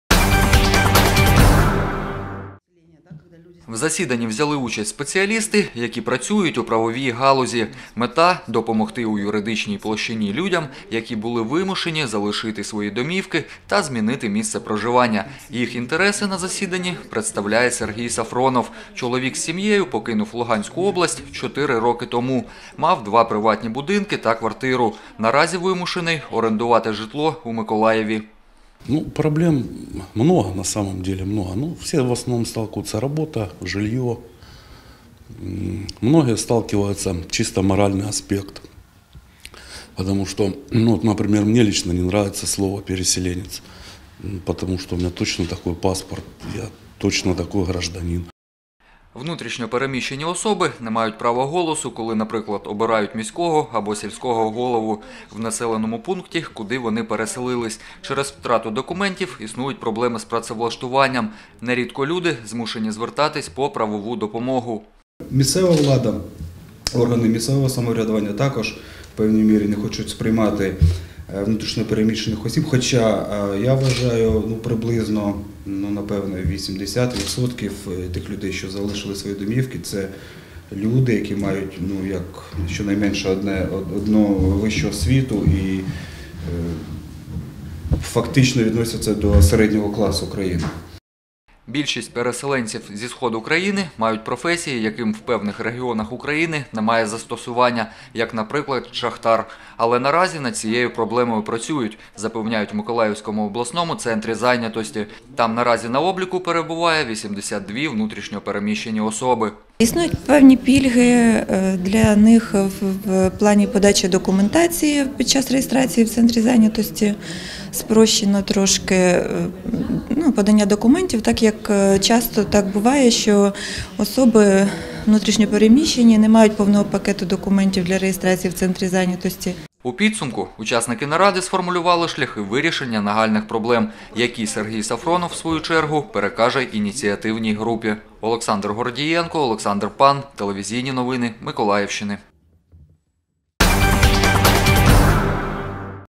ТНМ — сюжет. Юридична допомога внутрішньо переміщеним особам
Працевлаштування, правовий статус і житло. У миколаївському кризовому центрі за круглим столом обговорили перспективи вирішення нагальних проблем, з якими постійно стикаються внутрішньо переміщені особи.